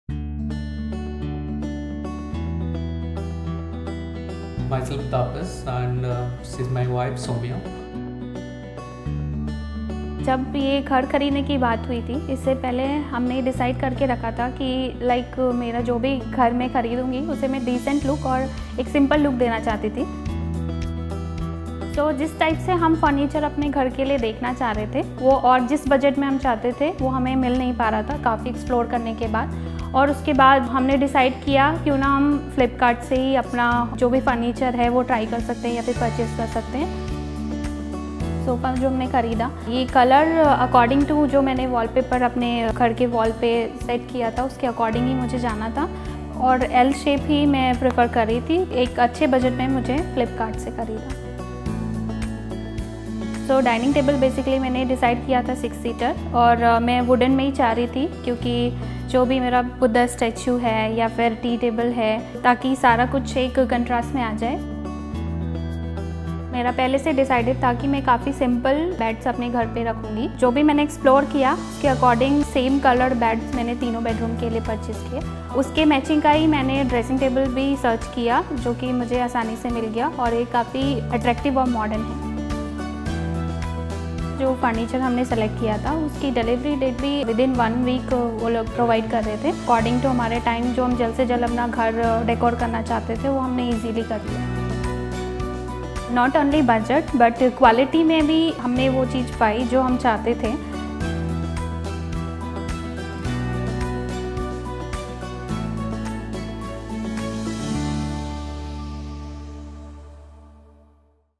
Real Homes, Real Stories with Flipkart Furniture - Tapas and Saumya
Check out how newly married Tapas and Saumya Chatterjee set up their brand new 3BHK Gurgaon home completely with Flipkart Furniture, all under Rs. 1 Lakh! With Durability Certified furniture at unbelievable prices, now you can too!
BeFurniSure with Flipkart - India's Largest Online Furniture Store.